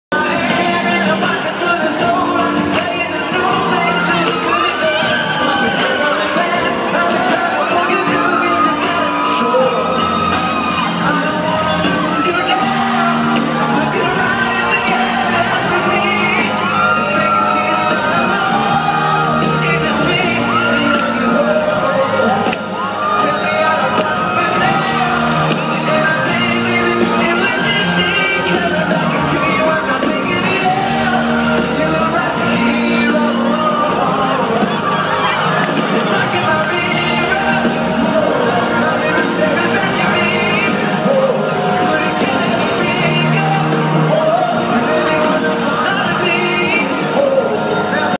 Gavin DeGraw (07/28/2013)
Gavin DeGraw hanging with us common folk at PNC Garden State Arts Center.. July 28, 2013